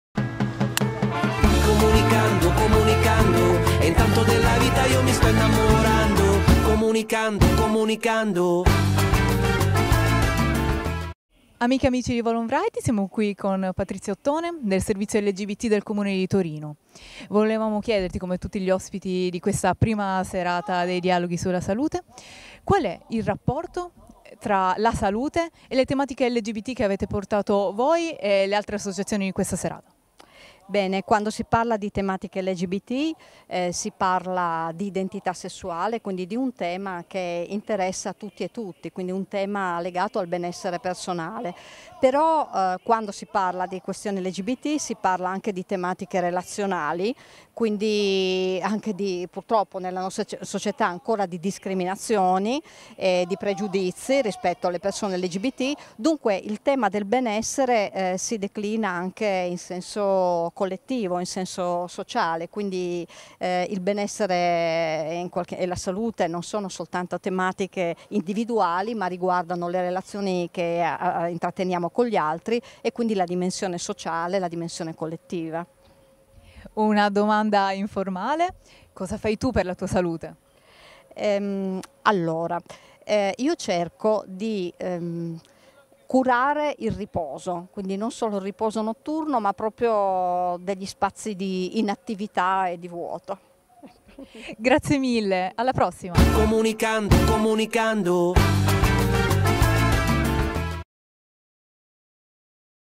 Dialoghi sulla Salute 2018 - Intervista a Patrizia Ottone
Intervista a Patrizia Ottone del Servizio LGBT della Città di Torino